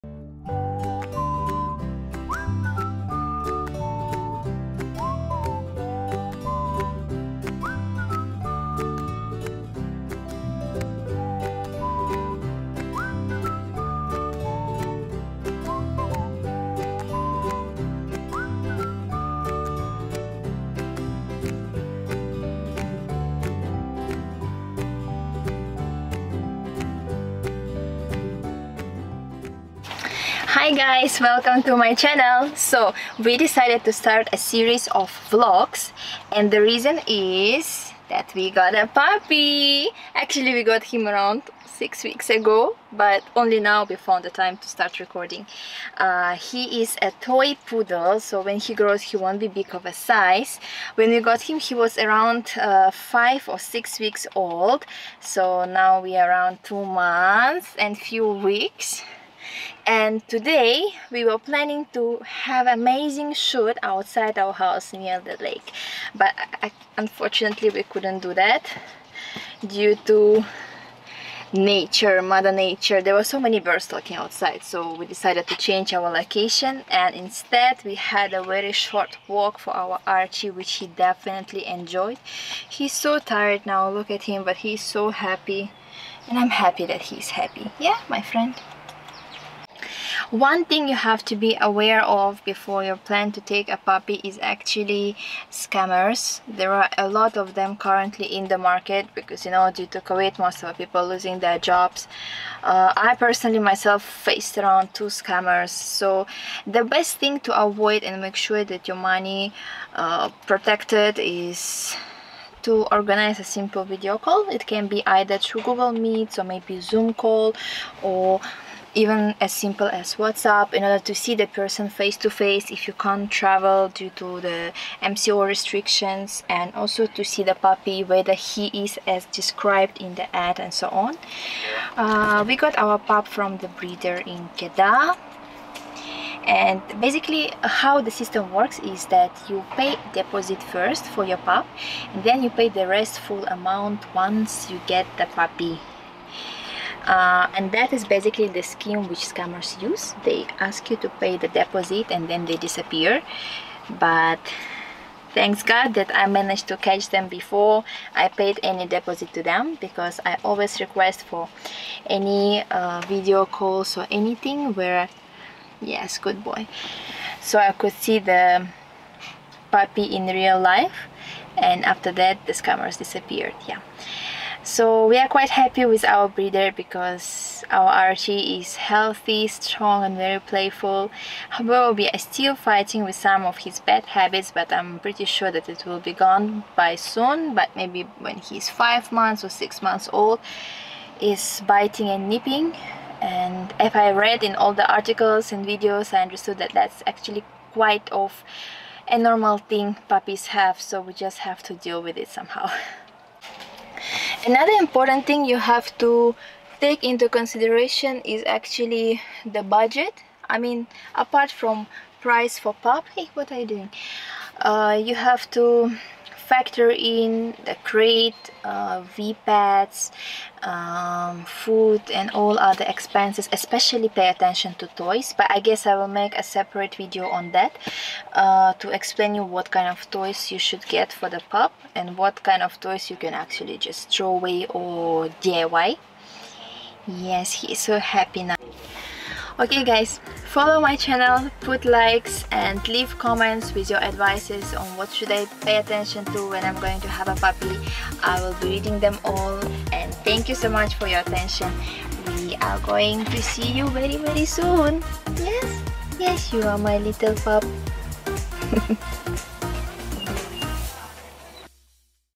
MEET MY NEW PUPPY!
I seriously cannot get over how tiny and cute he is here! he has grown so much since we got him!

I was so excited when filming this video and there's a lot of things I didn't exactly explain properly so I thought I would write them here.

A lot of you know that I have forever wanted a Toy Poodle, however, I needed to wait until we had the space for him in a large backyard and I had the time to train and walk him regularly. I have researched a lot about the breed and know they can be hard work as they are highly energetic dogs. Finally, we felt like we have everything ready for our new puppy!

Before finding where we got him from, I did look online and did some research to see if there were any Toy Poodle puppies that I was able to find. I did check in with local shelters from time to time for the year leading up to this. I just want to make it clear that if there was a puppy suitable for us, I would have adopted him 100%.

It was important to get a puppy rather than a full-grown dog, as he will adapt better to our environment. Being a first-time dog owner, I have done a lot of research which does say getting a puppy is a better decision as I don’t have a lot of experience with dogs. I am very proud to say I am a responsible pet owner and I will do everything I can to ensure when I bring an animal into my life, it is long-term. For so many reasons, adoptions or rescues don’t work out for those with existing pets or first-time owners and I didn’t want that to be us. Of course, we will consider adopting in the future, however, this decision to get Archi was one we waited a LONG time to actually do! This definitely doesn’t mean we won’t adopt in the future, but for right now, adopting wasn’t for us 💗

We decided to share our lifestyle with you as we got a puppy. It's our first dog and with zero experience in being a dog's parents let's embark on a cool journey together!

👉 Follow Archi & I on Instagram 😘

I can't believe we got a new puppy! We have always wanted to get a Dog but I had no idea my boyfriend was going to do this.